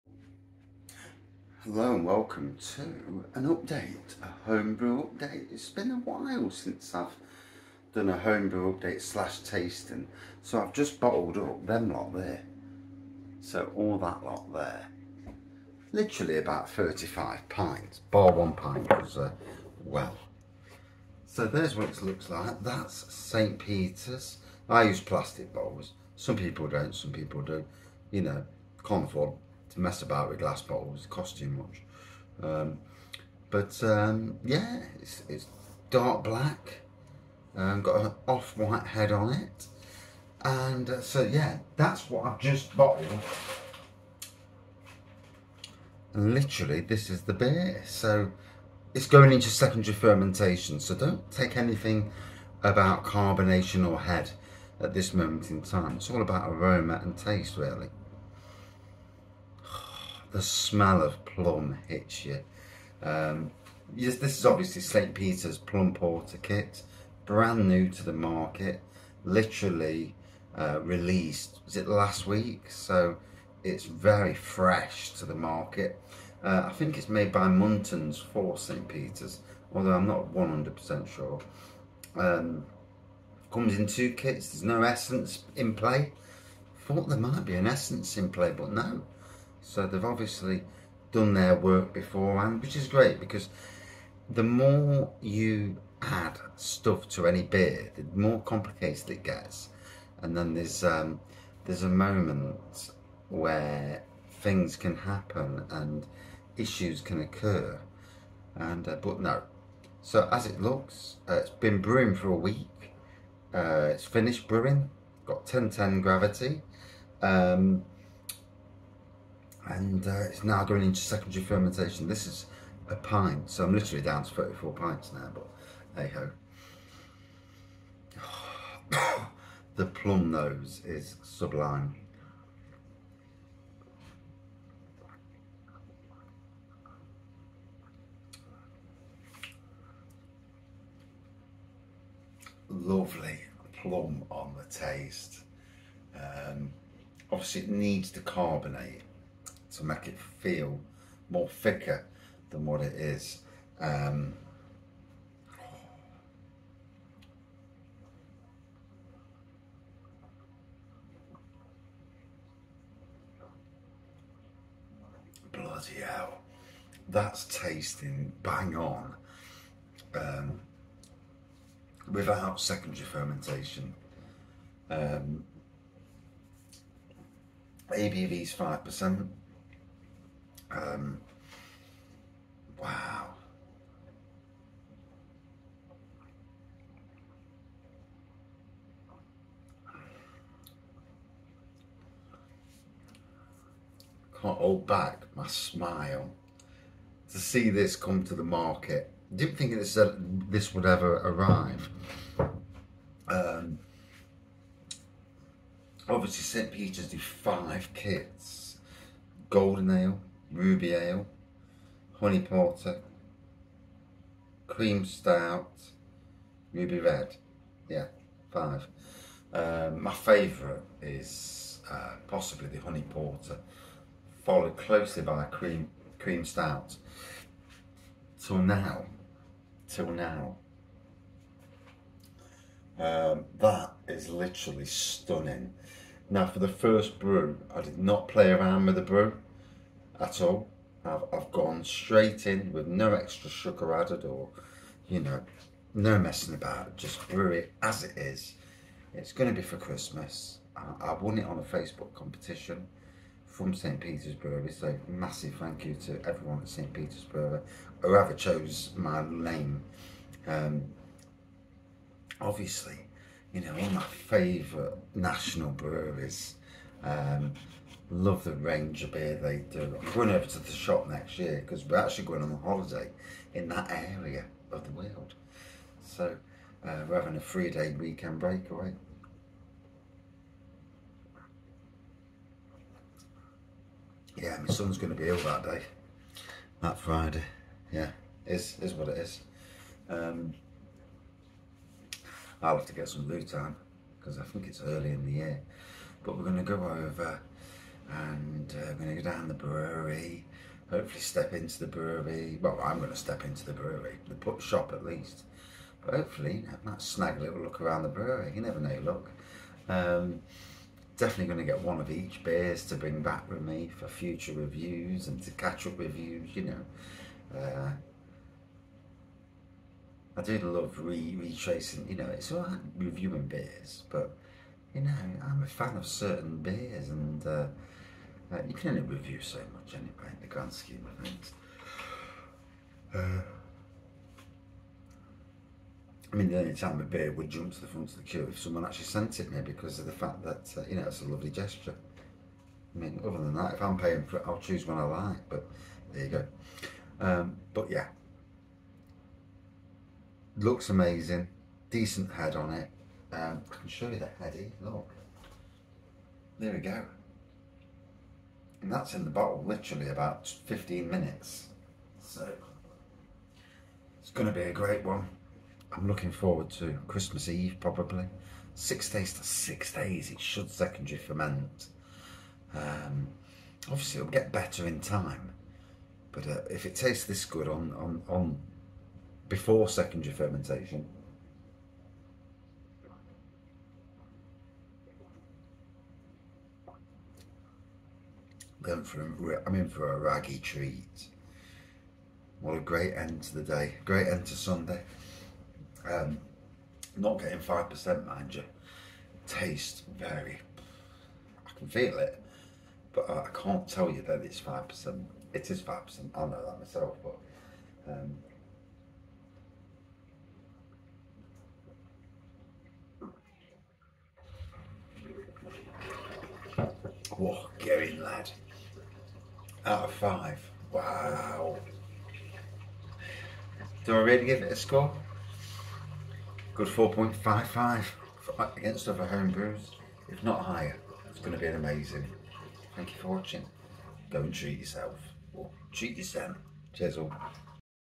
St Peters Brewery Homebrew Plum Porter Kit First Tasting and Review
For homebrewers like me this was the Beer Kit release of the year, been praying for a long time for this release, In this Video I've bottled the St Peters Plum Porter up and its the first tasting before 2nd fermentation.